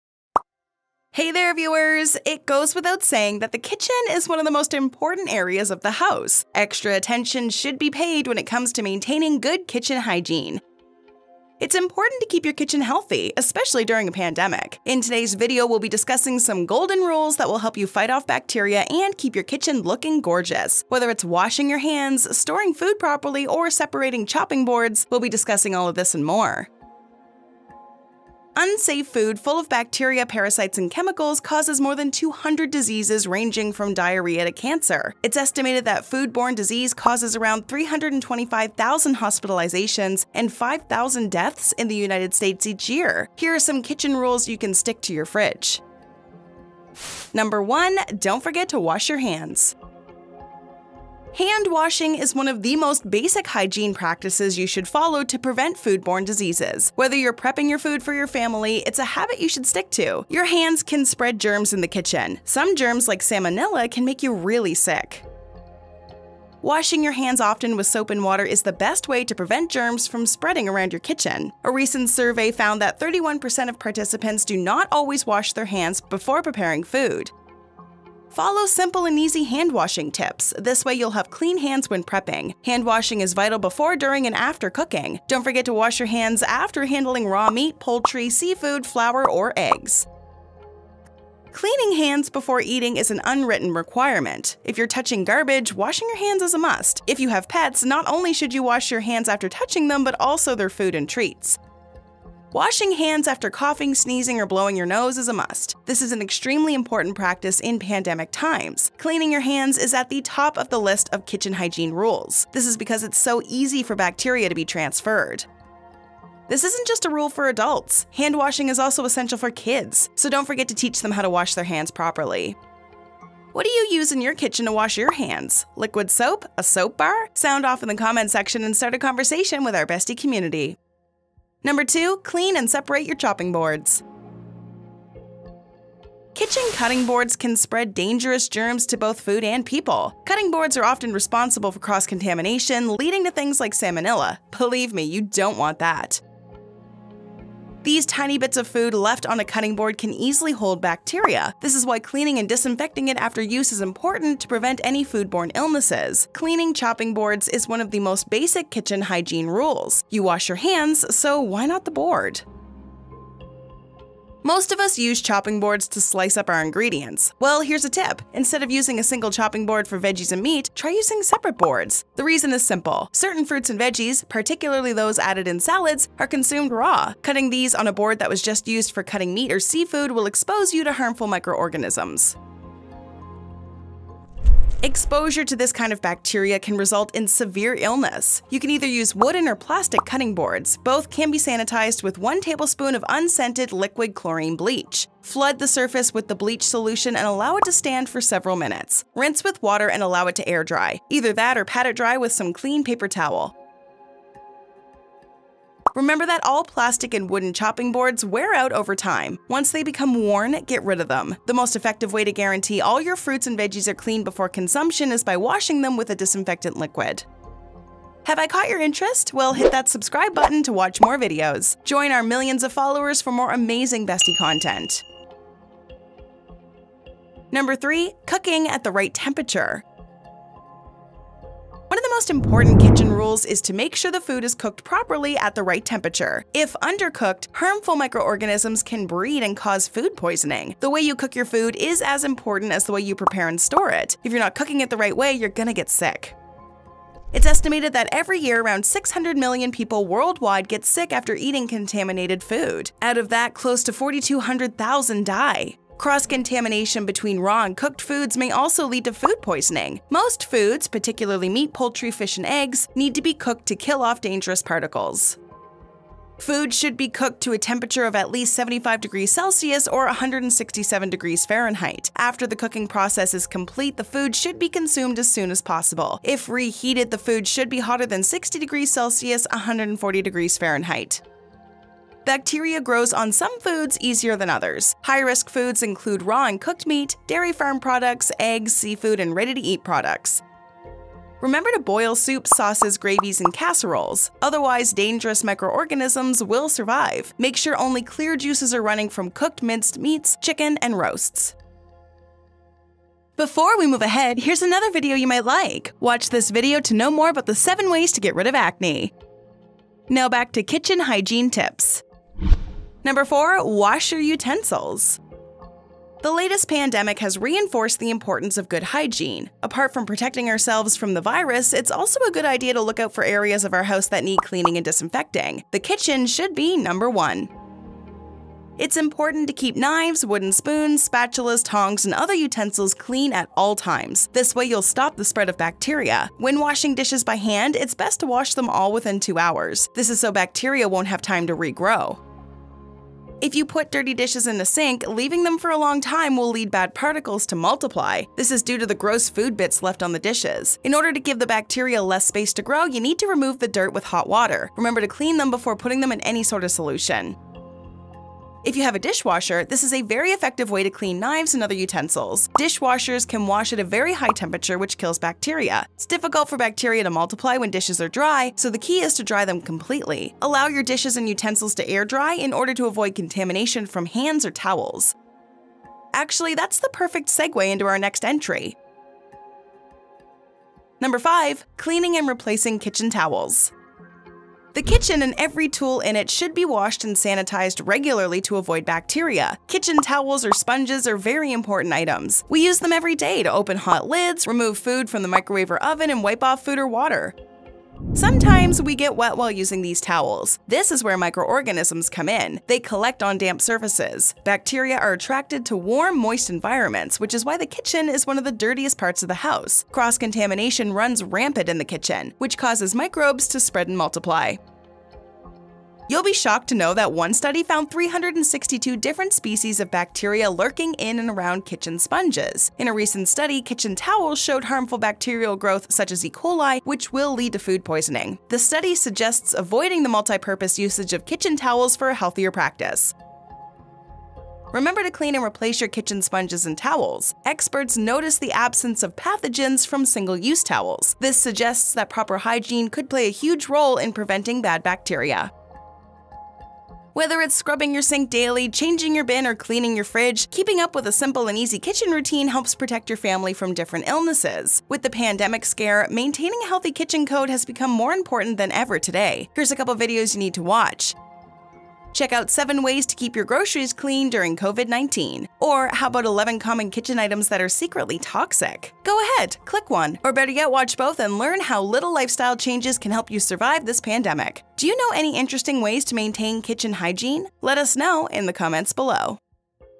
5 Ways To Keep Your Kitchen Germ Proof
In today’s video, we’ll be discussing some golden rules that will help you fight off bacteria, and keep your kitchen looking gorgeous. Whether it’s washing your hands, storing food properly, or separating chopping boards. We’ll be discussing all of this AND more...

Timestamps:
Intro -  00:00
Don’t Forget To Wash Your Hands - 00:47
Clean and Separate Your Chopping Boards - 02:11
Cooking At The Right Temperature - 03:47
Wash Your Utensils! - 05:13

Cleaning and Replacing Kitchen Towels - 06:21

Summary:
1. Don't Forget To Wash Your Hands
Hand washing is one of the most basic hygiene practices you should follow to prevent food-borne diseases. Whether you’re prepping food for you or your family, it’s a habit you should stick to. Your hands can spread germs in the kitchen. Some germs like salmonella can make you really sick.

2. Clean And Separate Your Chopping Boards
Kitchen cutting boards can spread dangerous germs to both food and people. Cutting boards are often responsible for cross-contamination, leading to things like Salmonella. Believe me, you don’t want that!

3. Cooking At The Right Temperature
One of the most important kitchen rules is to make sure the food is cooked properly at the right temperature. If undercooked, harmful microorganisms can breed and cause food poisoning. The way you cook your food is as important as the way you prepare and store it. If you’re not cooking the right way, you’re going to get sick.

4. Wash Your Utensils!
The latest pandemic has reinforced the importance of good hygiene. Apart from protecting ourselves from the virus, it’s also a good idea to look at areas of our house that need cleaning and disinfecting. The kitchen should be Number 1!